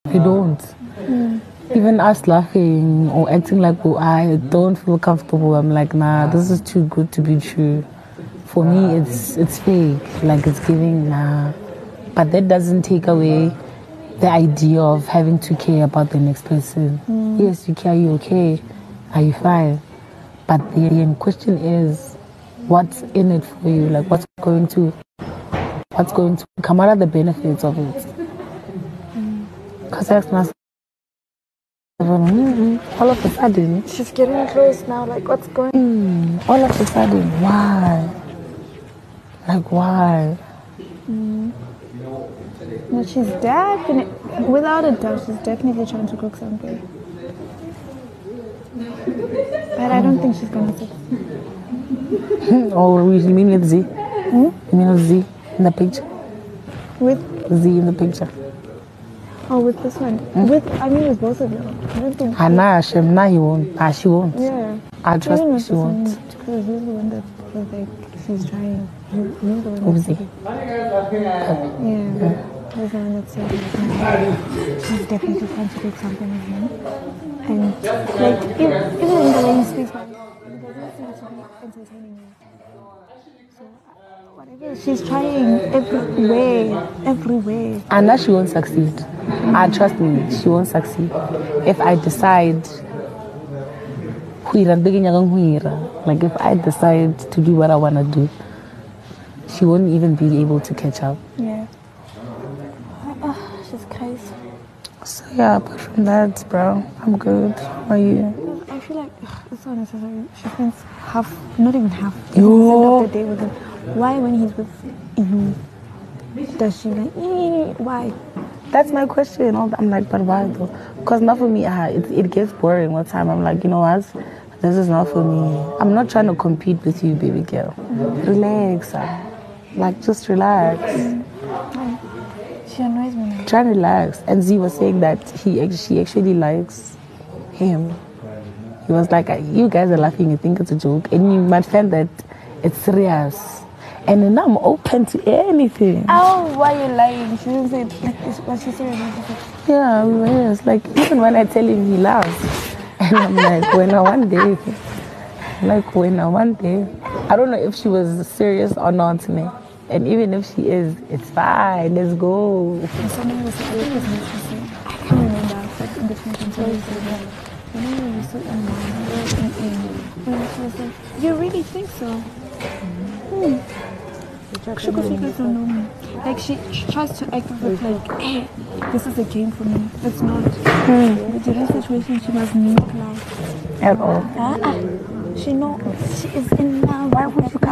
Mpumi : she’s not comfortable being around Liema as she tells Els
Mpumi says she is not comfortable being around Liema as  she and Els discuss their rectangle with Jareed.
Els also mentioned that it looks like Liema wants to "cook" something there by Young Pappi and Mpumi mentioned that Zee said he likes Jareed.

This Triangle is damn longggggg😂🤣🤣